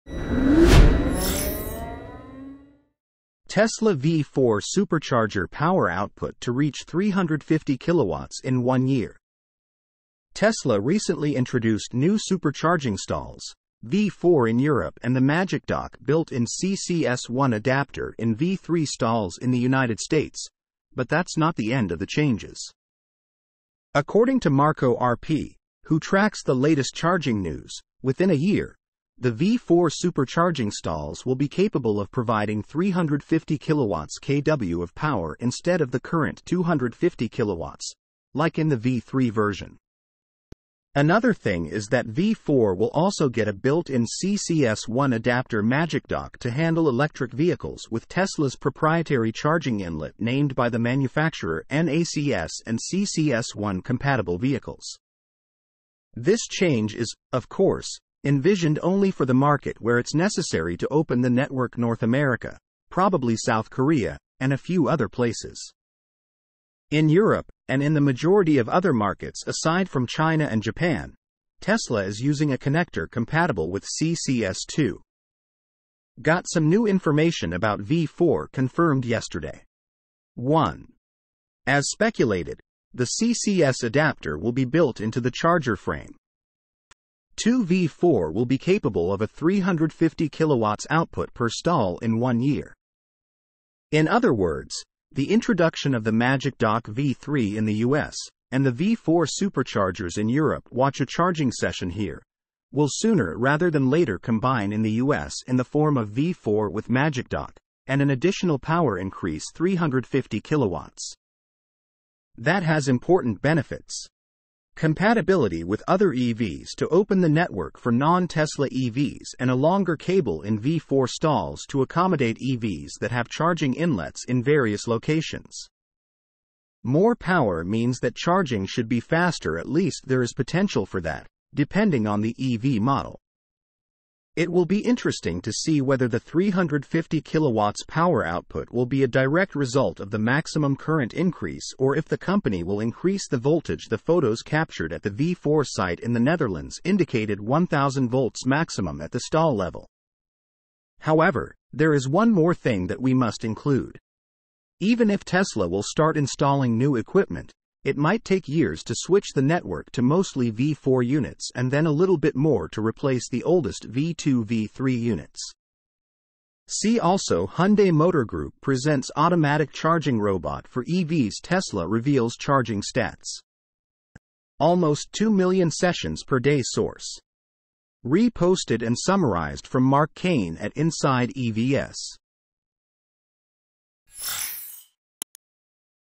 Tesla V4 Supercharger Power Output To Reach 350 kW In 1 Year
Tesla recently introduced new Supercharging stalls (V4 in Europe) and the Magic Dock (built-in CCS1 adapter in V3 stalls in the United States), but that's not the end of the changes.

Source: Re-posted and Summarized from Mark Kane at insideevs.com.